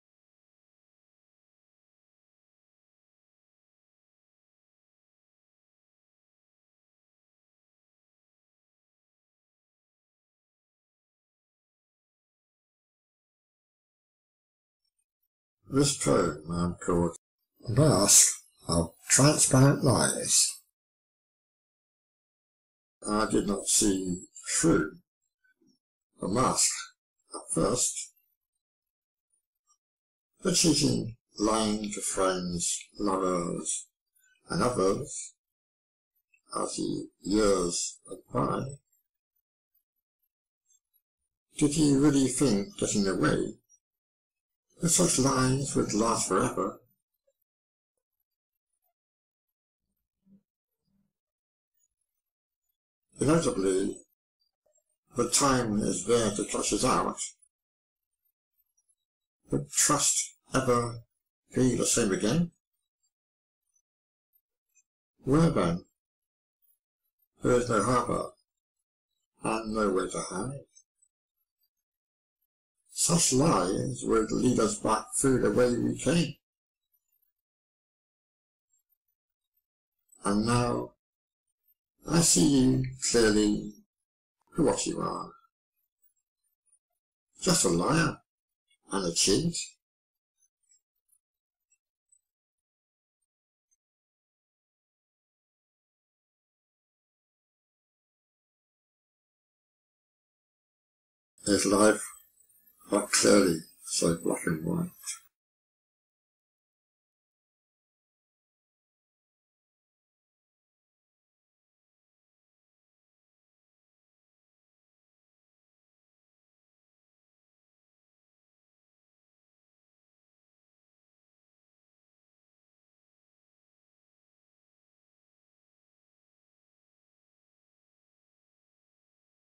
Poem and Painting Mask of Transparent Lies Written, Painted and Presented by Christopher Englis
DescriptionPoem and Painting  - Mask of Transparent Lies - Written, Painted and Presented by Christopher English

This painting shows a story:

A sailor arrives home to greet his lover on the seashore harbour, he is sure of himself hidden behind a mask, he holds flowers in his hands to hide a lie. In his boat he hides another lover, she is hidden behind a sail that is also a veil; both women are pregnant. The boat mast becomes a gallows; this is where the sailor hangs himself (metaphorically) when he has nowhere to hide after his secret becomes clear.